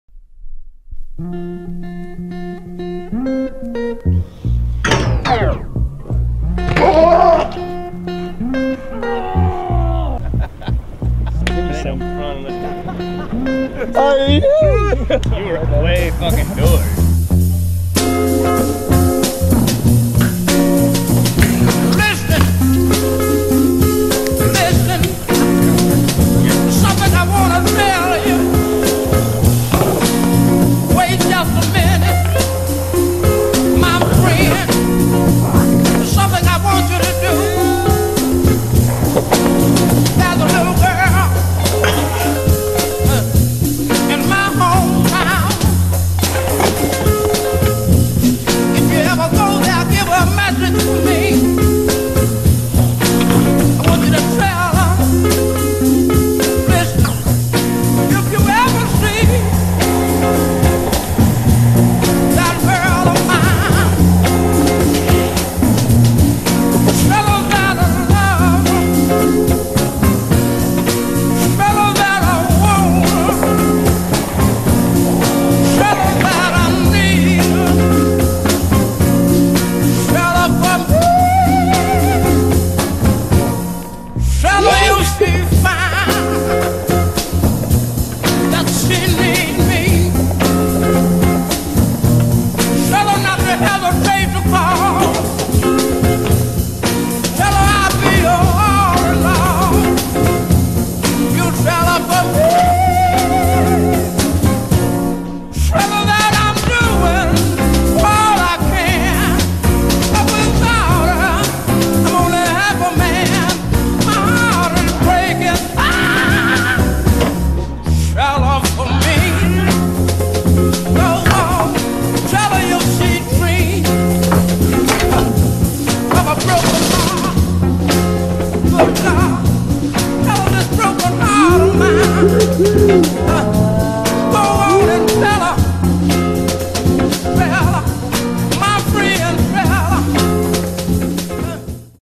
Verde BMX: Tony Neyer & Drew York in Long Beach
Tony Neyer and Drew York go hard in Long Beach for Verde BMX